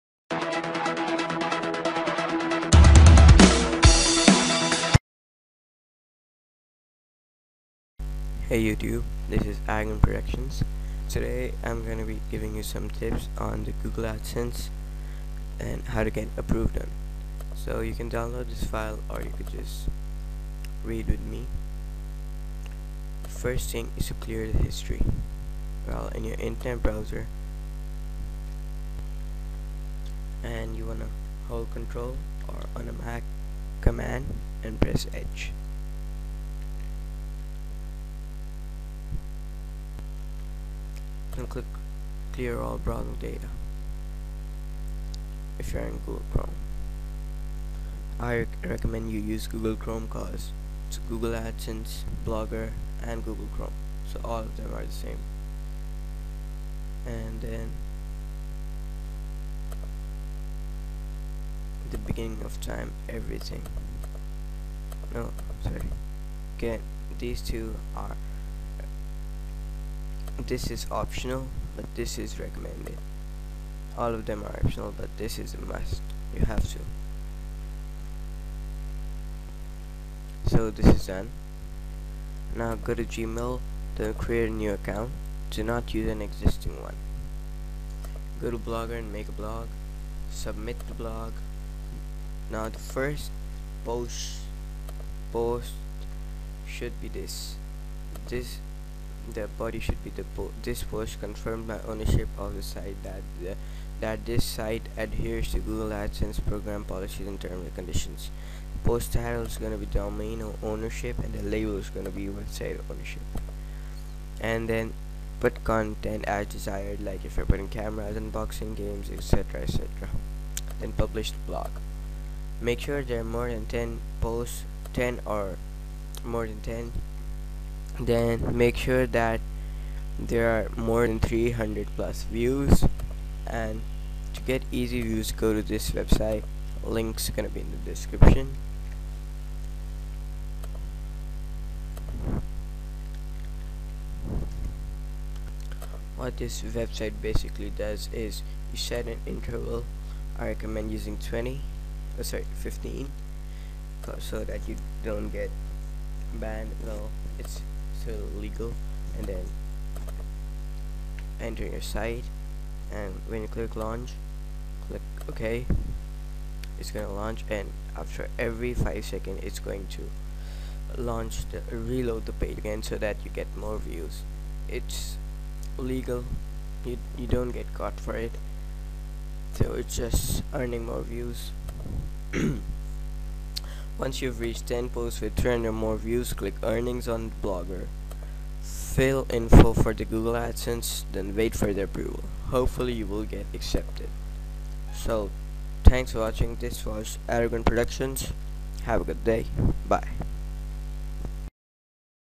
Google AdSense Approval Tips 2012 [HD]
I decided not to put a download link, instead the whole text
Google Adsense Approval Tips
1)Clear The History
3) Create New acc (Dont use existing)
4) Go to blogger and make a blog
5) Submit blog

Blog  Details As Followed:
This Post confirms my ownership of the site and that
post title:               domain ownership
label:                      website ownership

6)Put content as desired (EG: Cameras, Unboxings,Games, etc etc)
7)Publish it
8)Make sure that there are more than 10POSTS
9)Make sure that there are more than 300+ Views
11) Once reached 10posts with 300 or more views click earnings on blogger
12) Fill in info for Google Adsense
13) Wait for approval!  [Hope fully you will get accepted]